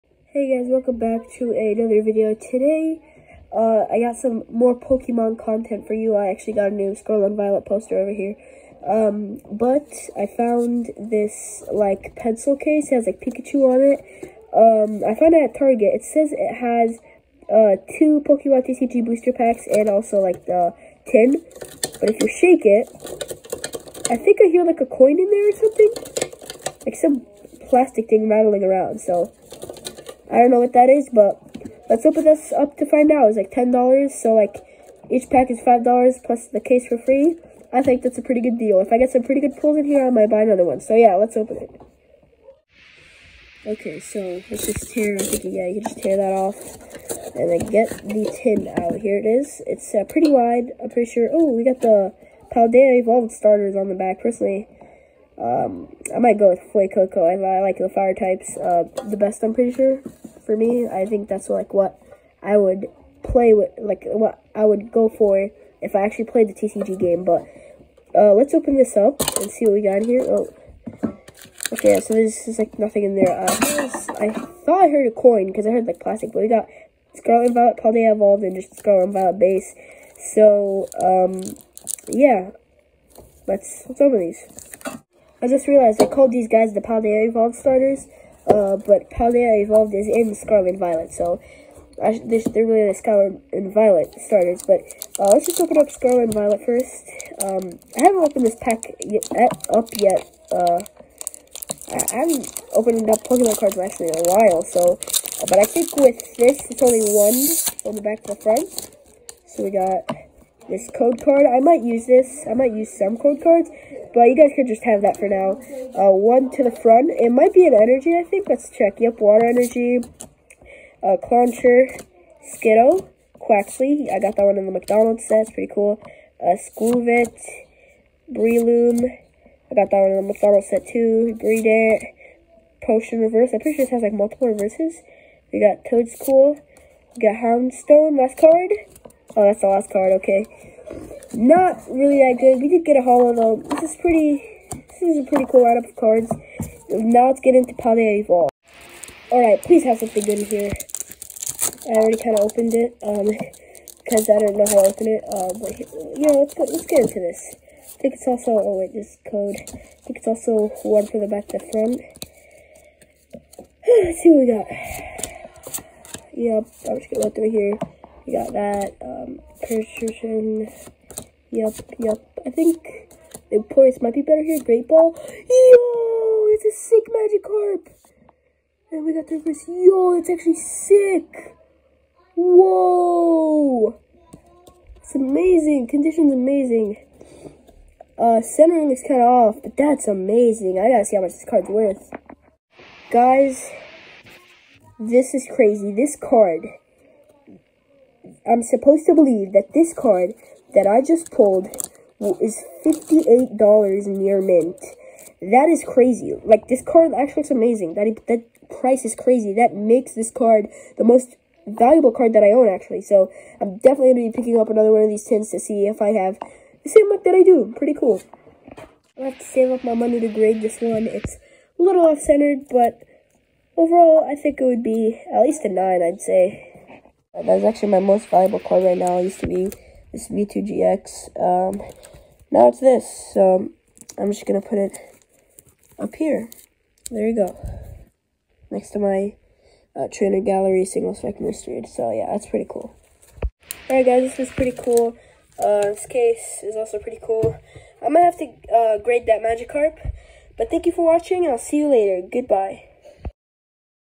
Opening Pikachu pencil tin (CRAZY PULL)
I opened this Pokémon Pikachu pencil tin on my birthday and got a crazy card!